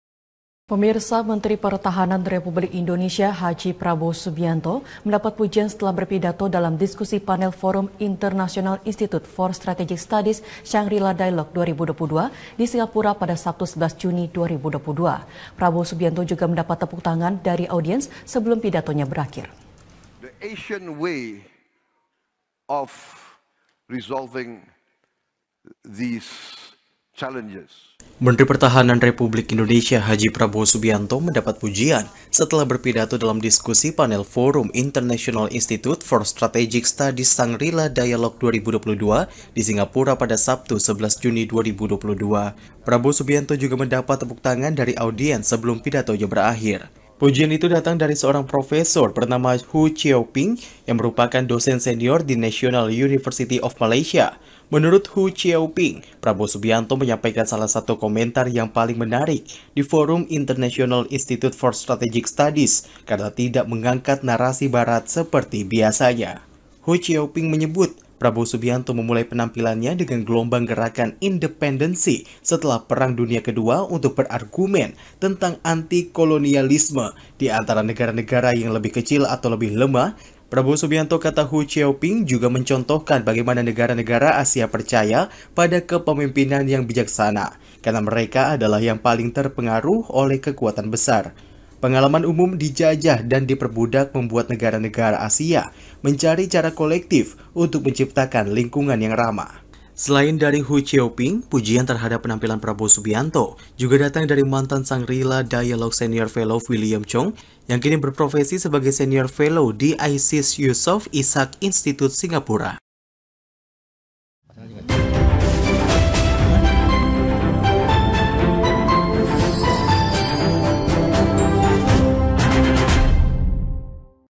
Pidato Prabowo Subianto di IISS Shangri La LA Tuai Pujian
Menteri Pertahanan Republik Indonesia Prabowo Subianto mendapat pujian setelah berpidato dalam diskusi panel forum international institute for strategic studies Shangri-La dialogue 2022 di Singapura pada Sabtu 11 Juni 2022. Prabowo Subianto juga mendapat tepuk tangan dari audiens sebelum pidatonya berakhir.